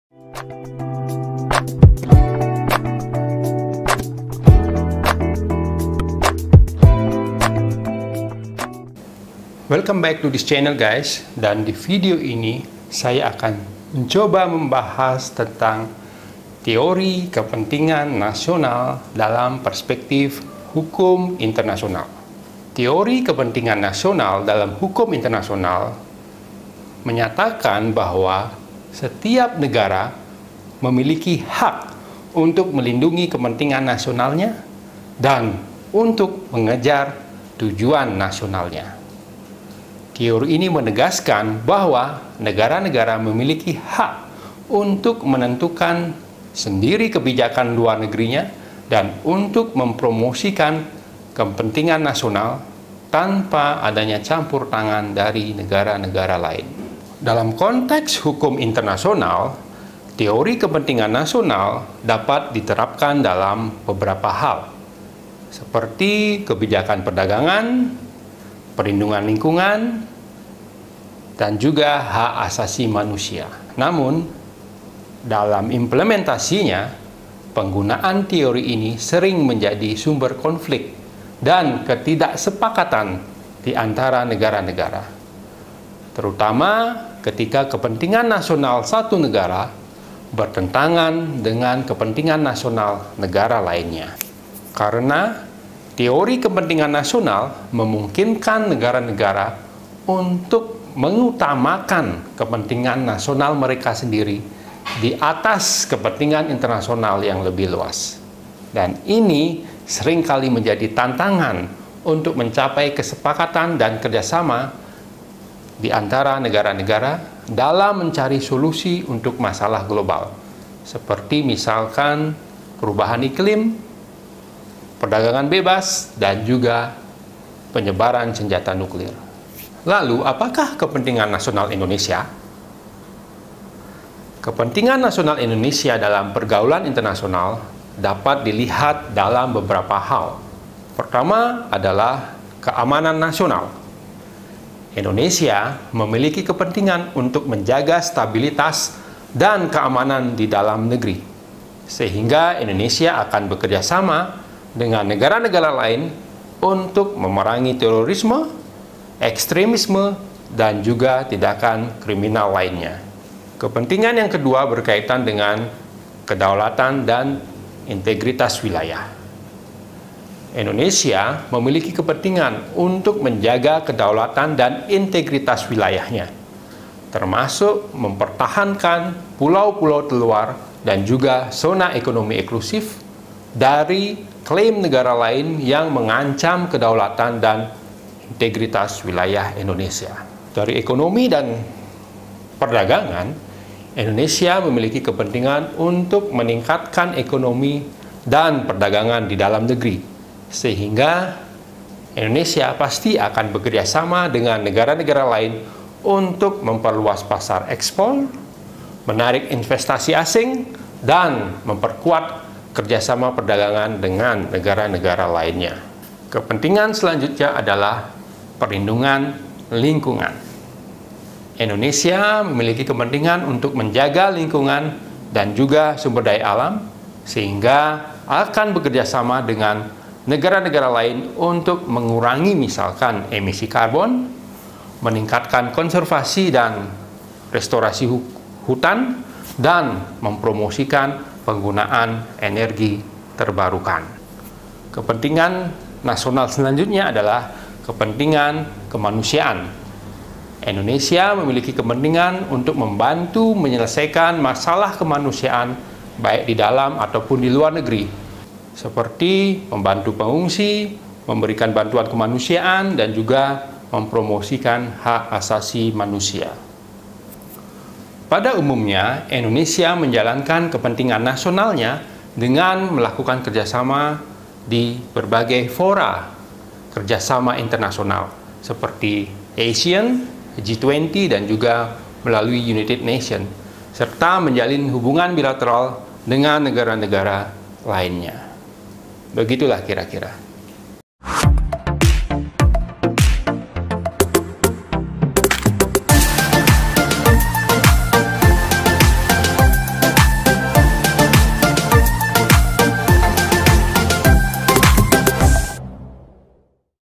Mengenal Teori Kepentingan Nasional dalam Hukum Internasional
Welcome back to this channel guys dan di video kali ini saya akan membahas tentang teori kepentingan nasional dalam perspektif Hukum Internasional.

Teori kepentingan nasional dalam hukum internasional mengemukakan bahwa setiap negara memiliki hak untuk melindungi kepentingan nasionalnya dan mengejar tujuan nasionalnya. Teori ini menegaskan bahwa negara-negara memiliki hak untuk menentukan sendiri kebijakan luar negeri dan untuk mempromosikan kepentingan nasional mereka tanpa campur tangan dari negara-negara lain.

Dalam konteks hukum internasional, teori kepentingan nasional dapat diterapkan pada berbagai hal, seperti kebijakan perdagangan, perlindungan lingkungan, dan hak asasi manusia. Namun, dalam prakteknya, penggunaan teori ini sering menjadi sumber konflik dan ketidaksepakatan antara negara-negara, terutama ketika kepentingan nasional satu negara bertentangan dengan kepentingan nasional negara lain.

Lalu bagaimana dengan Kepentingan Nasional Indonesia? Tonton video lengkapnya ya guys.